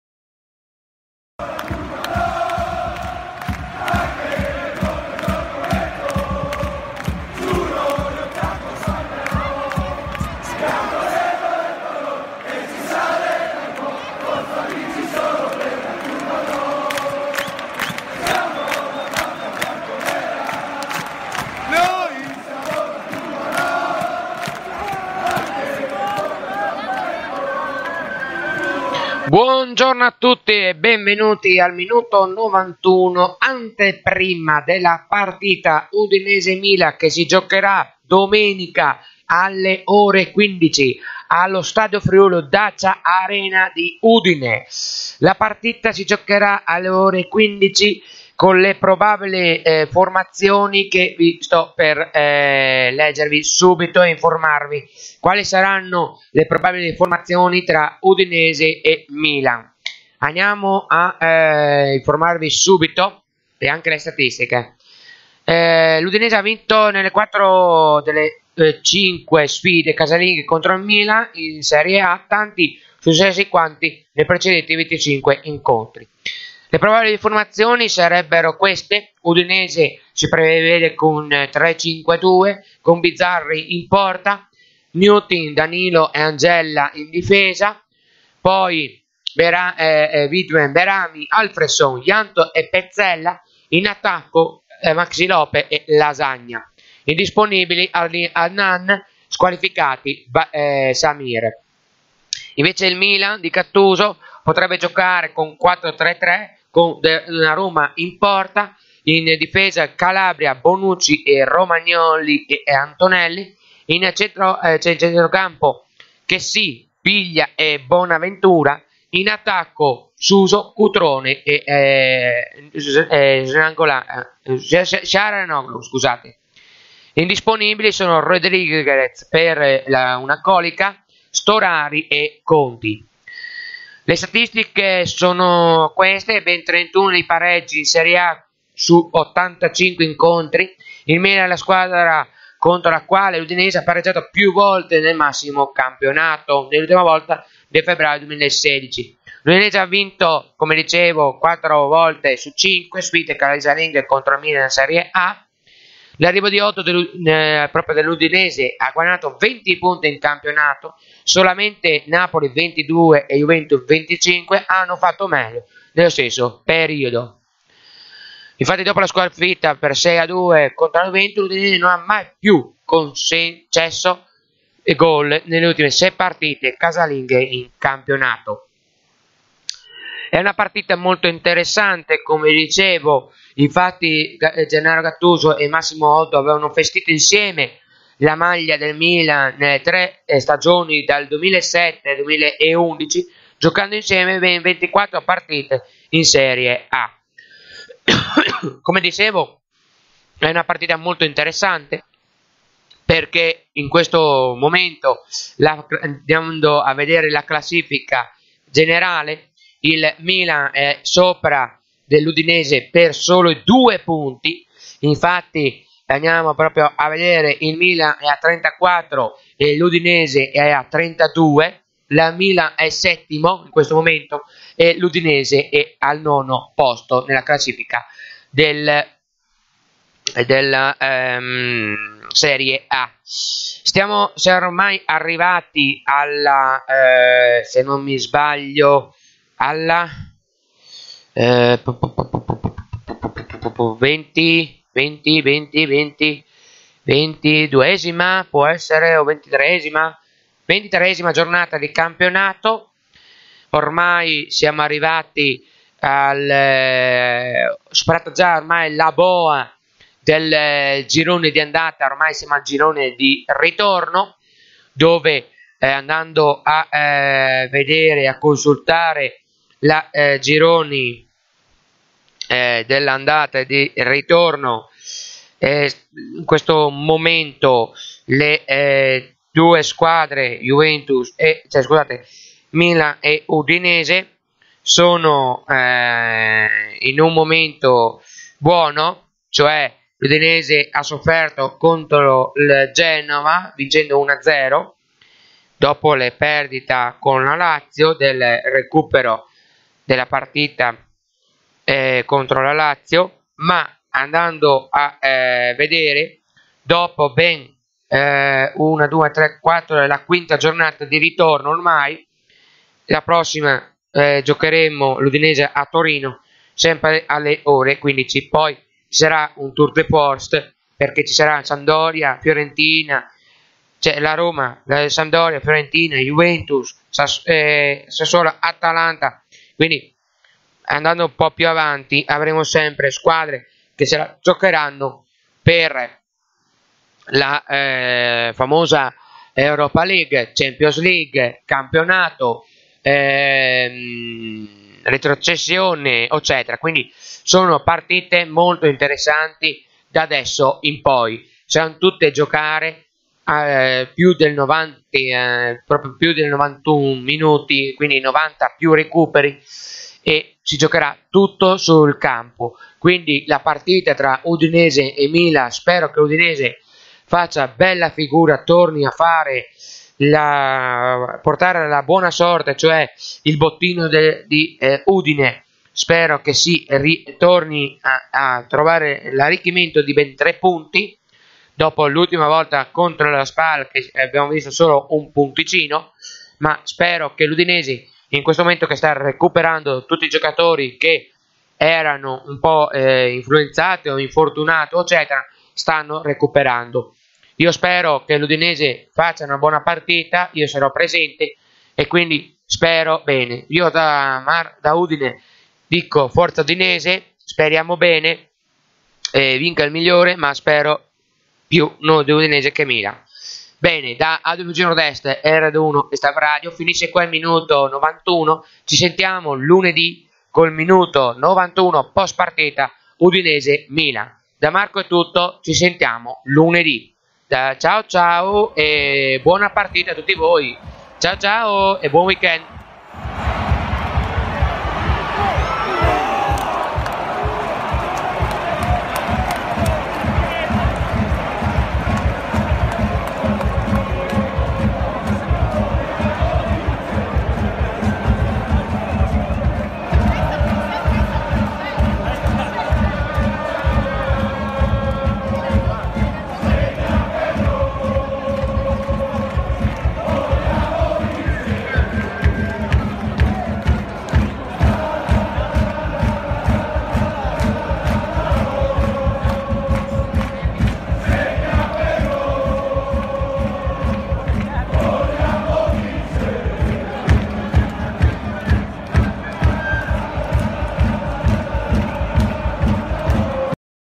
Min91 Ante Udinese-Milan di Marco da Udine
Descrizione della ventitresima partita di campionato dell'udinese 2017/18 e non solo, da Marco di Audio Radio FVG NordEst, Rdaradio1 e Stapradio